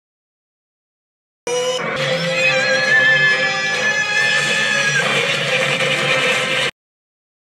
My Reaction That O.K.K.O. Let’s Be Heroes is Getting Cancelled after the last 2 Episodes!
NOOOO! I LIKE THAT SHOW, ITS MY 3rd FAVORITE CARTOON NETWORK SHOW!
R.I.P. O.K.O! Let’s be Heroes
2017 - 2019
Note: I like Strawberry Shortcake: Itty Bitty Adventures!